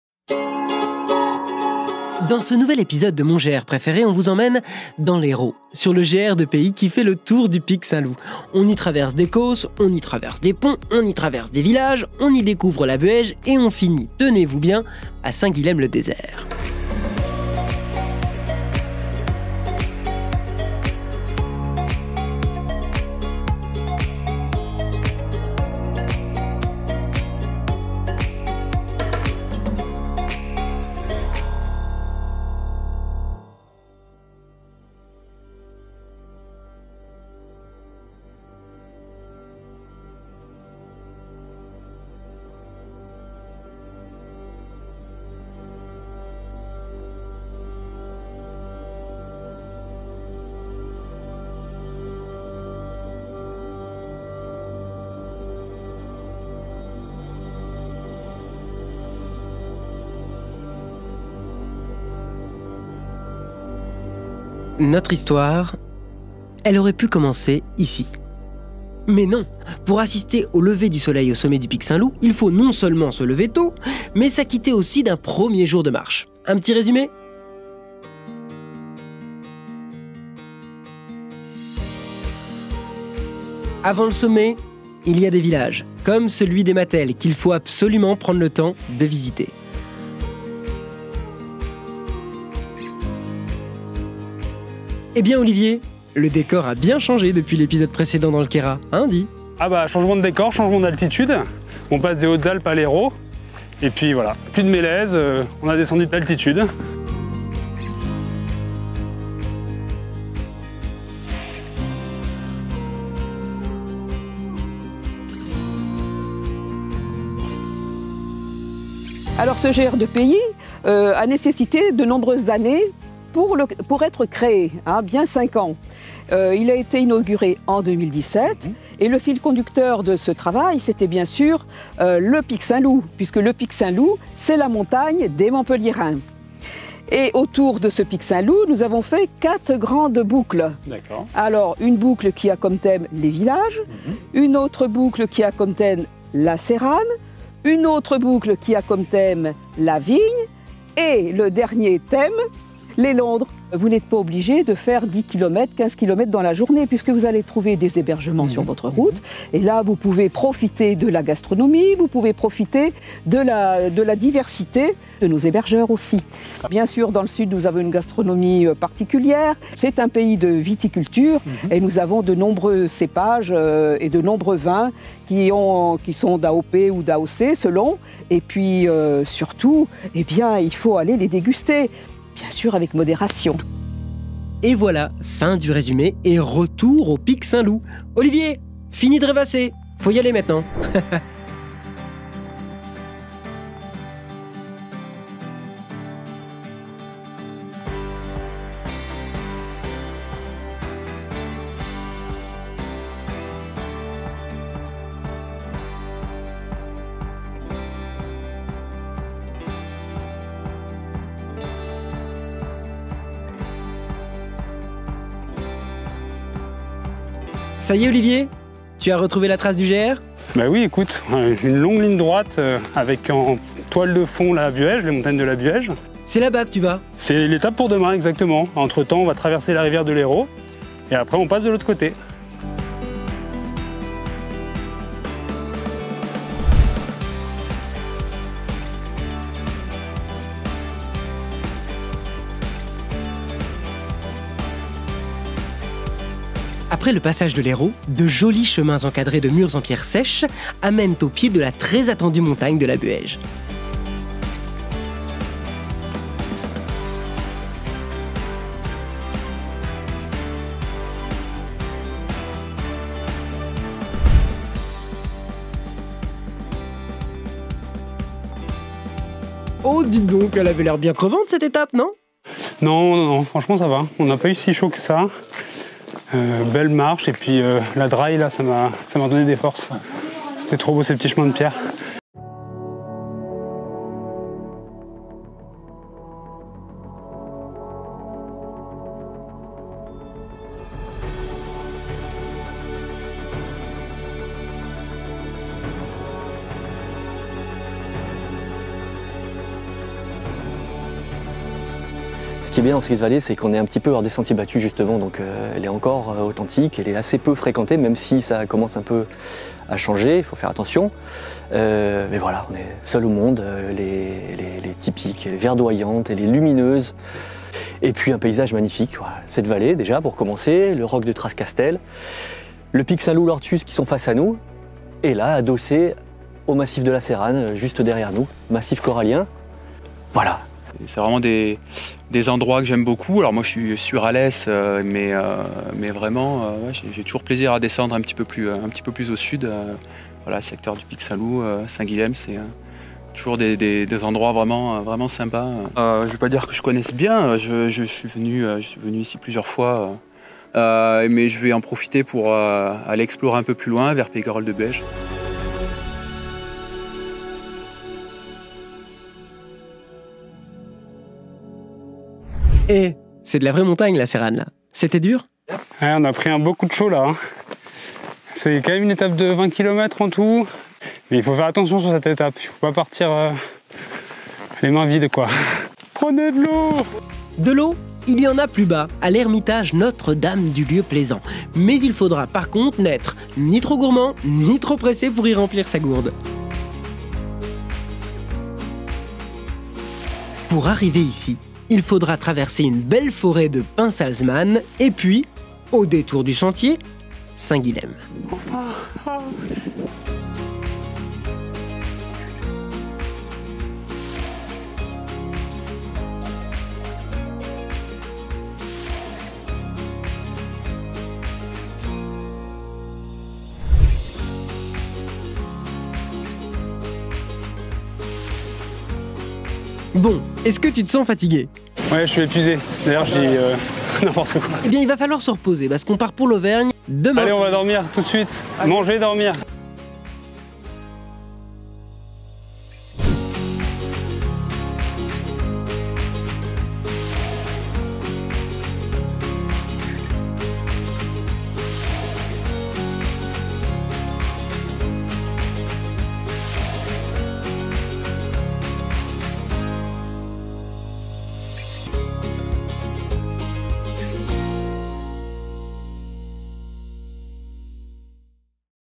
Le Grand Tour du Pic St-Loup, mon GR® Préféré ?
Pour la 4e saison, la Fédération Française de Randonnée Pédestre organise le concours de "Mon GR® Préféré".
En 2020, le GRP du Grand Pic St-Loup était en lice... et a remporté le concours de Mon GR® Préféré, grâce à vos votes.

Retrouvez-nous :
Cette vidéo vous a plu ?
Recevez tous nos coups de cœur et nos idées de séjours de weekend ou d’escapades dans l’Hérault